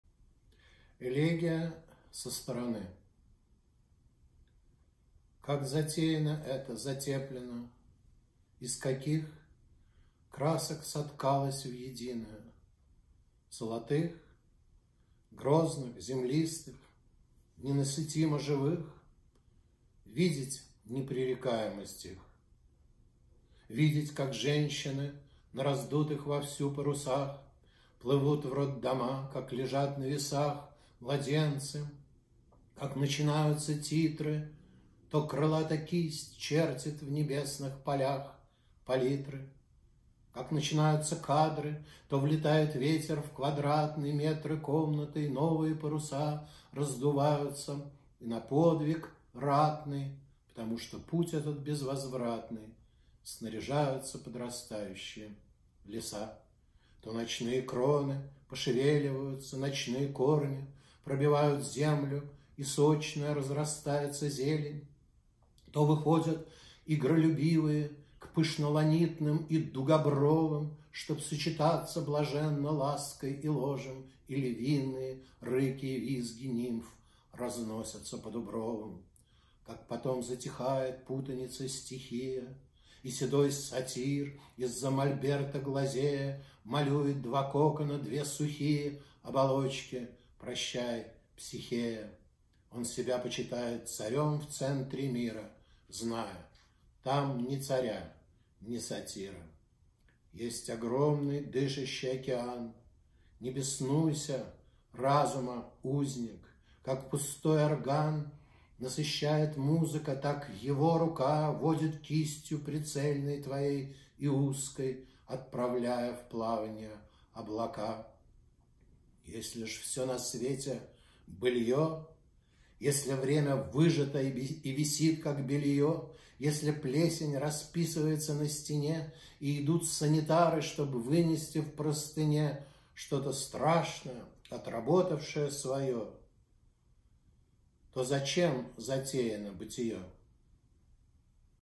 Элегия Со стороны
Как затеяно это, затеплено, из каких
красок соткалось в единое – золотых?
грозных? землистых? ненасытимо-живых?
Видеть непререкаемость их.

Видеть, как женщины на раздутых вовсю парусах
плывут в роддома, как лежат на весах
младенцы, как начинаются титры –
то крылата кисть чертит в небесных полях палитры, –

как начинаются кадры –
то влетает ветер в квадратны
метры комнаты, и новые паруса
раздуваются, и на подвиг ратный
(потому что путь этот – безвозвратный)
снаряжаются подрастающие леса.

То ночные кроны
пошевеливаются, ночные корни
пробивают землю,
и сочная разрастается зелень.

То выходят игролюбивые
к пышноланитным и дугобровым,
чтоб сочетаться блаженно лаской и ложем, и львиные
рыки и визги нимф разносятся по дубровам.

Как потом затихает путаница-стихия,
и седой сатир, из-за мольберта глазея,
малюет два кокона, две сухие
оболочки, – прощай, Психея!

Он себя почитает царём в центре мира,
зная: там ни царя, ни сатира.

Есть огромный дышащий океан.
Не беснуйся, разума узник.
Как пустой орган
насыщает музыка,
так Его рука
водит кистью прицельной твоей и узкой,
отправляя в плаванье облака.

Если ж всё на свете быльё,
если время выжато и висит, как бельё,
если плесень расписывается на стене
и идут санитары, чтоб вынести в простыне
что-то страшное, отработавшее своё,
то зачем затеяно бытиё?